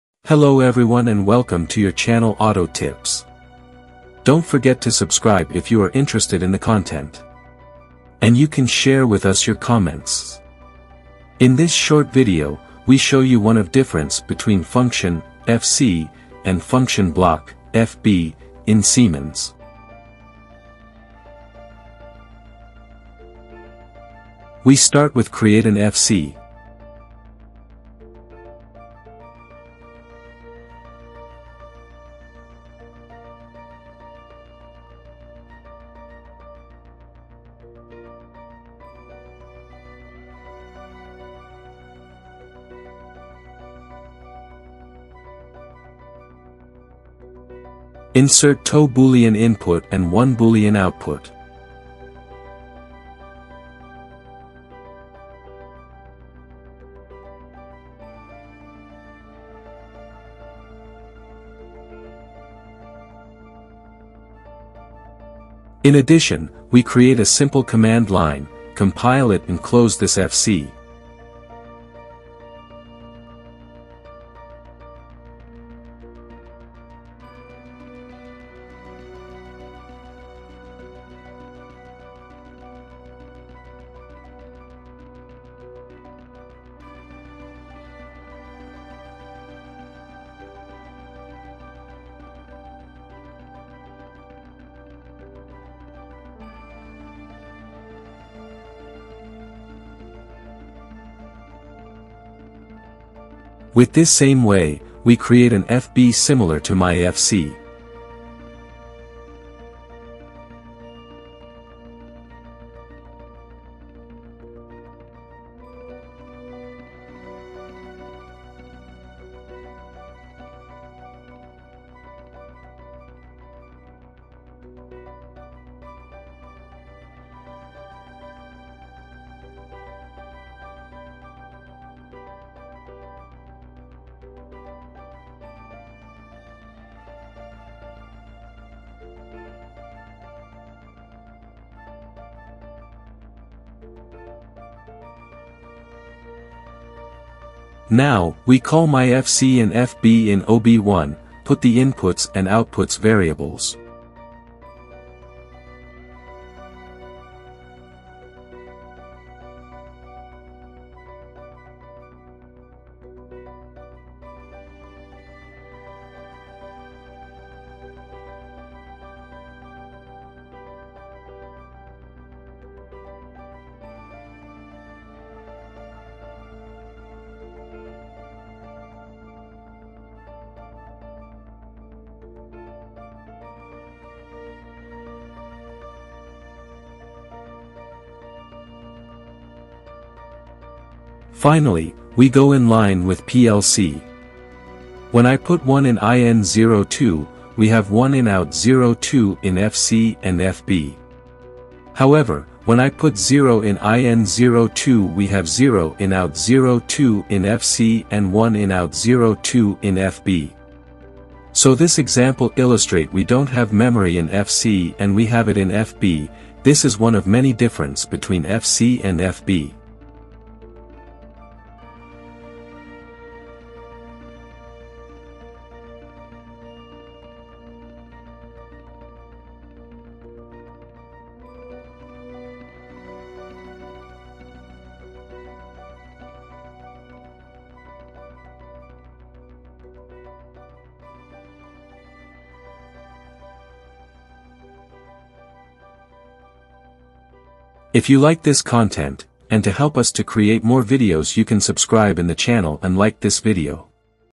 FC & FB : YOU KNOW THIS DIFFERENCE !
One of many difference between FC and FB in SIEMENS.